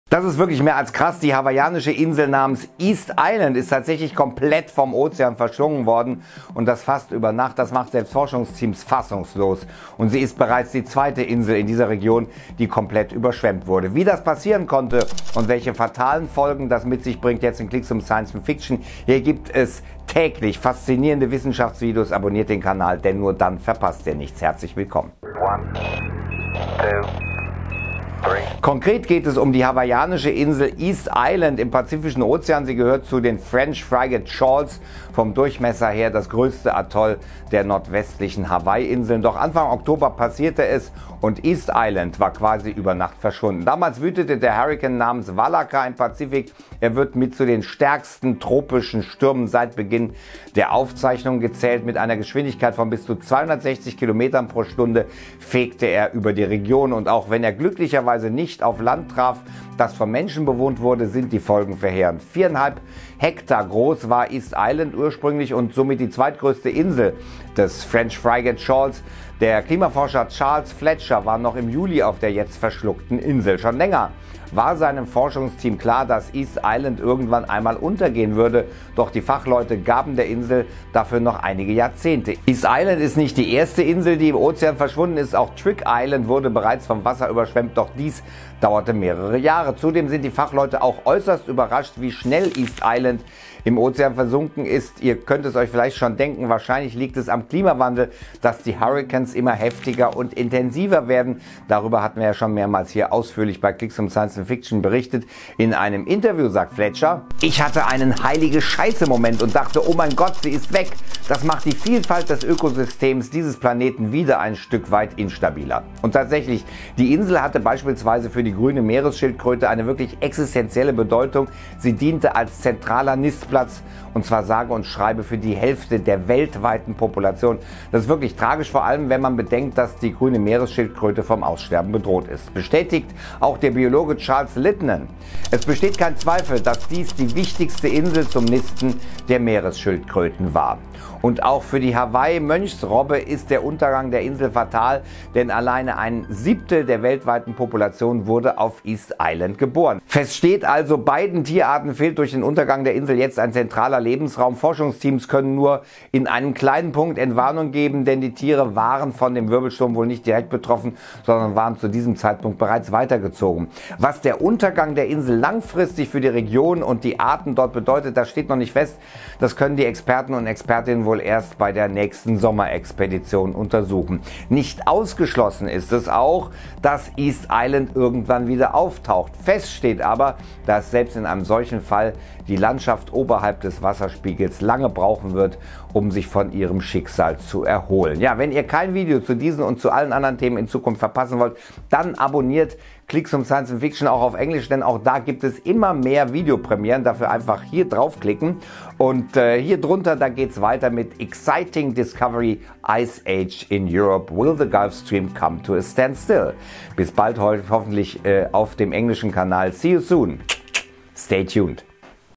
Klimawandel schlägt zu: Komplette Insel vom Ozean verschlungen!
Die hawaiische Insel namens “East Island” ist vom Ozean verschlungen worden. Und das fast über Nacht. Das macht selbst Forschungsteams fassungslos. Und sie ist ist bereits die zweite Insel in dieser Region, die komplett überschwemmt wurde. Ist das Verschwinden auf den Klimawandel zurückzuführen?

Schnitt: Vincent Viebig